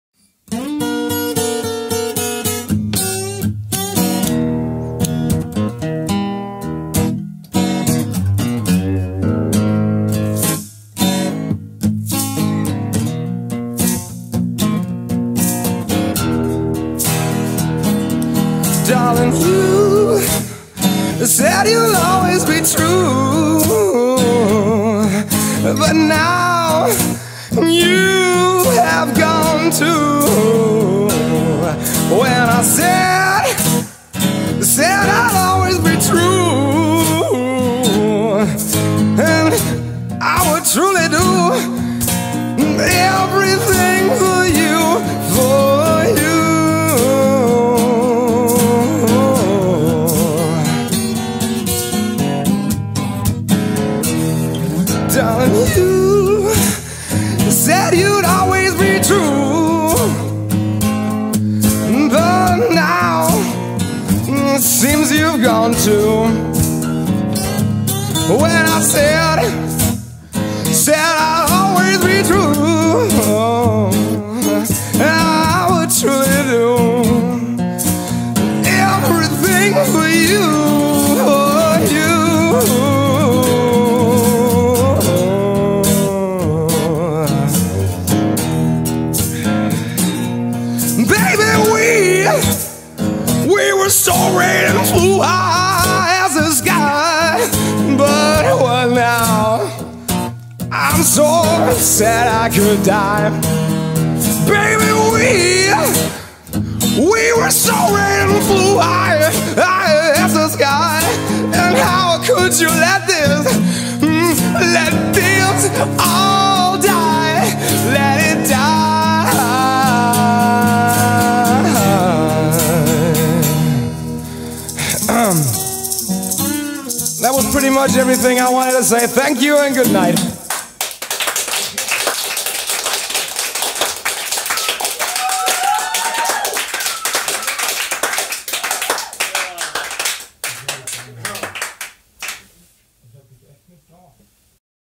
Deathless Creation - Funny Outro Idea To End A Record
Just messing around with an untuned guitar (on purpose), happened on the spot, the handclaps are real. There were a bunch of girls drinking Prosecco in the studio... It was fun, don't take it too serious.